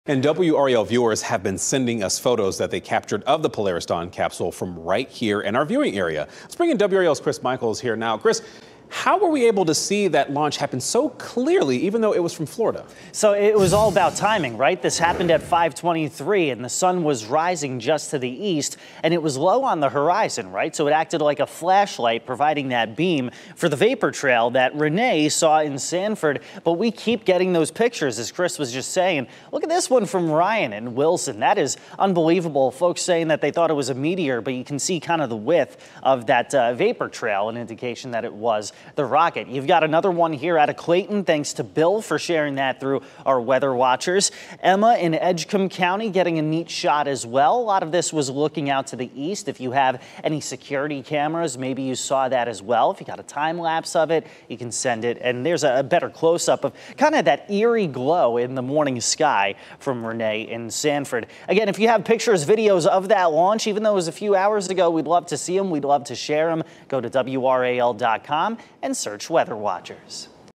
A look at how the SpaceX flight was visible from NC
WRAL meteorologist Chris Michaels talks about the Polaris Dawn launch and how we were able to see it in North Carolina.